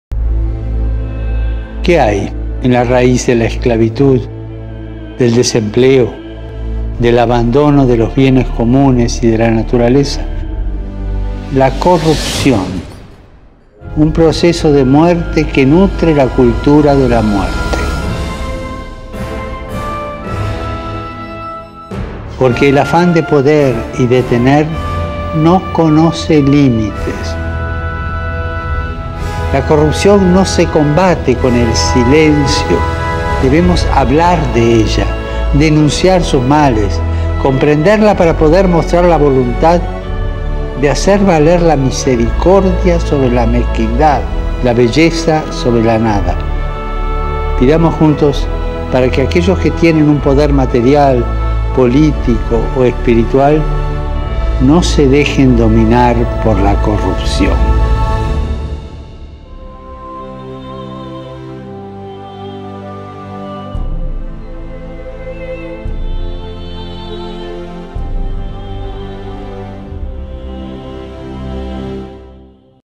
A pápa videója - 2018. február - A korrupció ellen
A korrupció ellen: Hogy akiknek anyagi, politikai vagy spirituális hatalmuk van, ne hagyják befolyásolni magukat a korrupciótól.